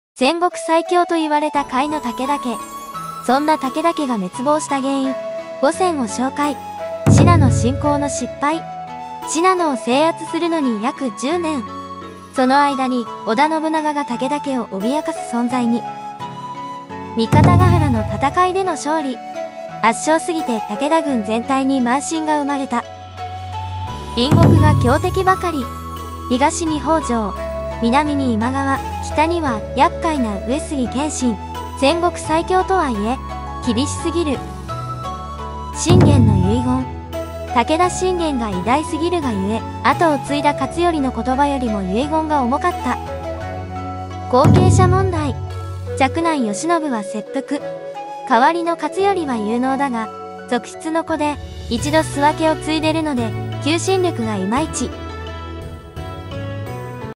【５つの原因】武田家滅亡の真相に迫る！その背後にあるものとは？武田家滅亡
武田家滅亡の５つの原因を解明し、その背後について